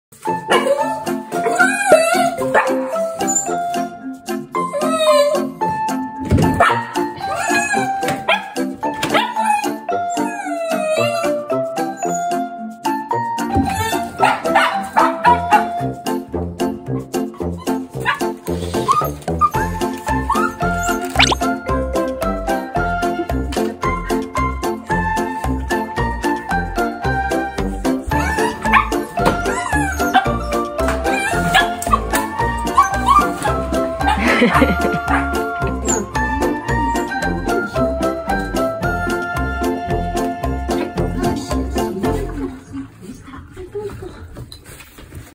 【高速しっぽ】すごい勢いでお隣さんちに侵入する犬【シュナウザージジトト】
お隣さんちはパパのご実家です。
たぶん不法侵入ではないと思われます。

お義母さんはわんちゃんが大好きで、
かわいいのでついおやつをあげてしまうので、
ジジトトも餌付けされてしまった様子なのでした。

最近はジジトト健康管理のためおやつは禁止中です。
可愛いからと言っておやつのあげすぎは気をつけましょう。

Miniature schnauzer with daughter
Jiji & Toto & Musume

ジジ/Jiji  6歳のころ（B&S）
トト/Toto  4歳のころ（S&P）